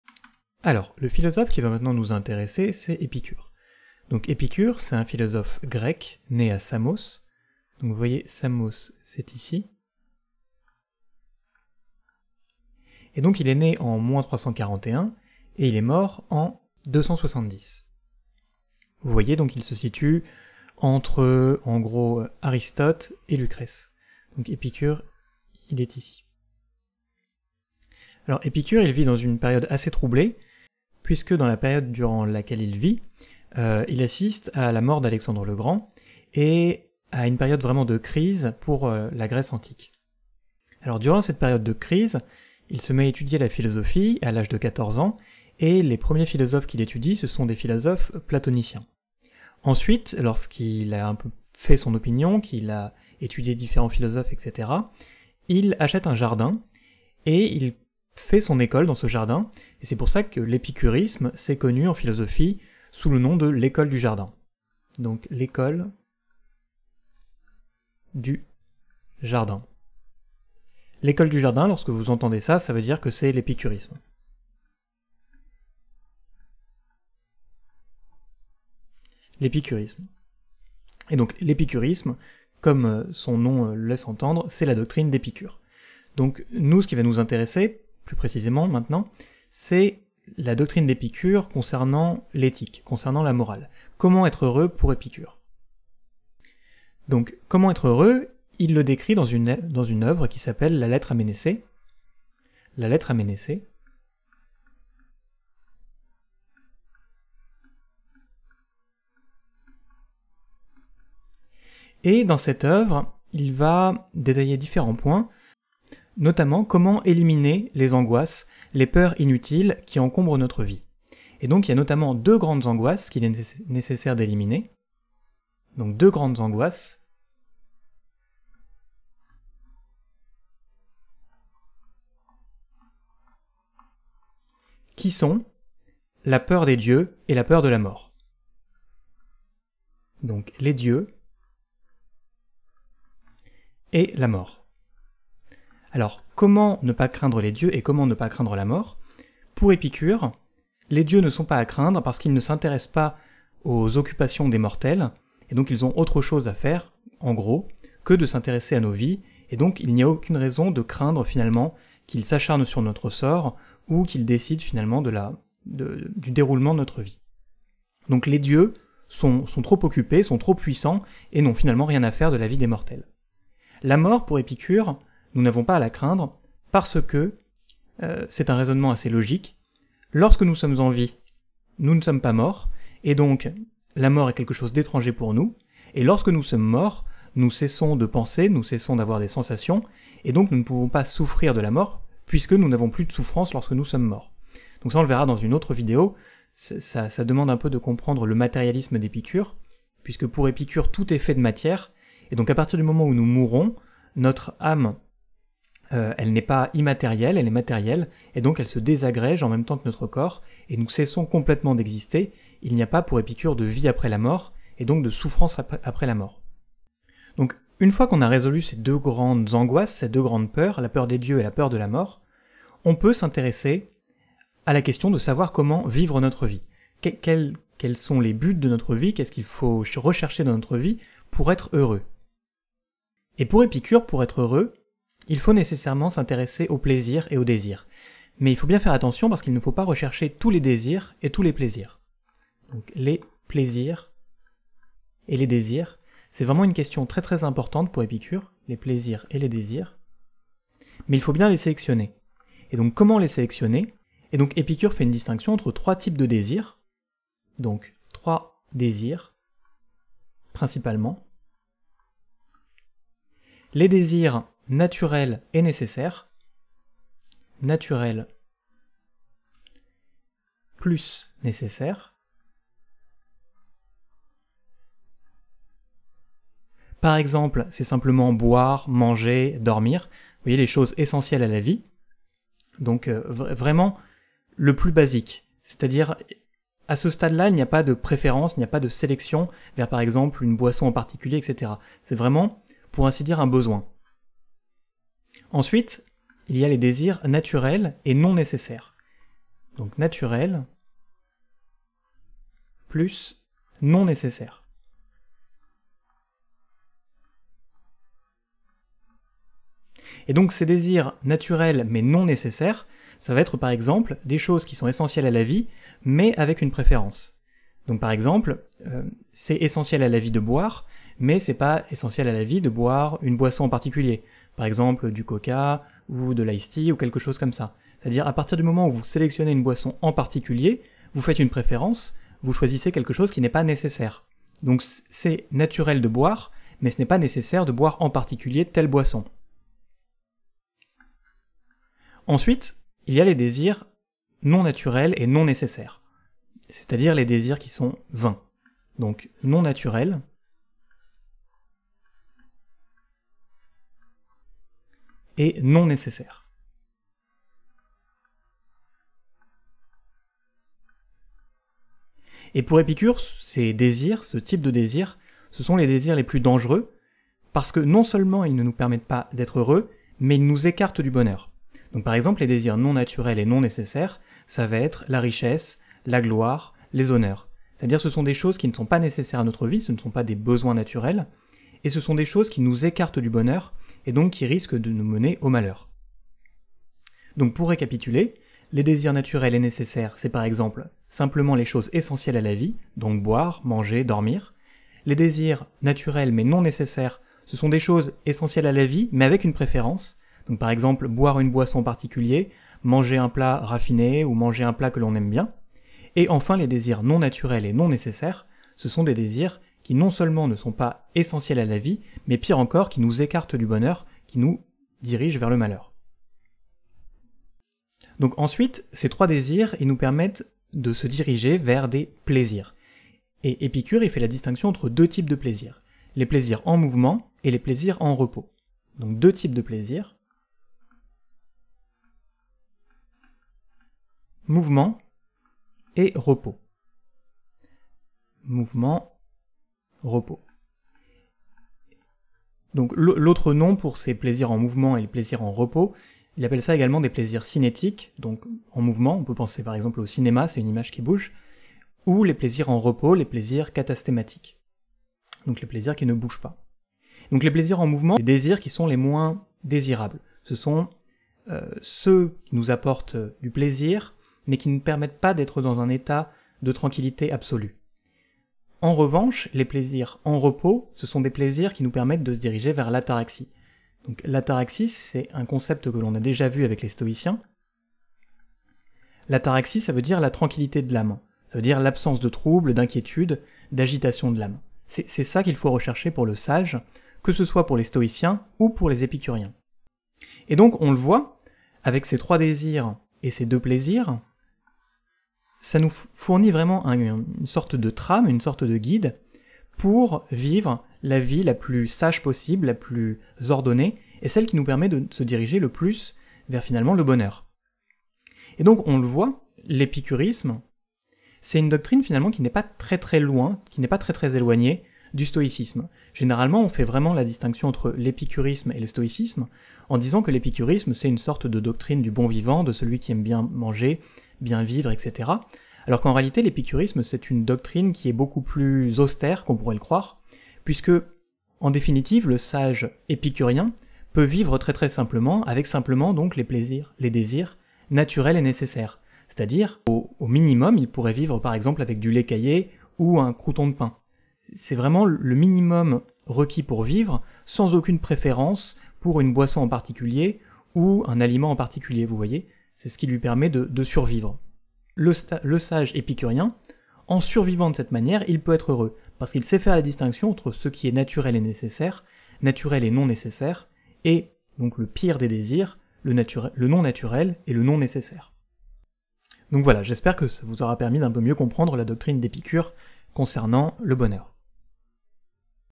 Philosophie - Comment être heureux selon Épicure ?
Retrouvez-moi en live sur Twitch Dans cette vidéo nous allons étudier la partie éthique de la philosophie d'Épicure en étudiant la distinction entre les désirs naturels et nécessaires, les désirs naturels et non nécessaires et les désirs non naturels et non nécessaires.

Références :
Philosophie, les auteurs, les oeuvres, Jacqueline Russ, éd. Bordas.
Manuel Philosophie Terminale, éd. Nathan. (frise chronologique)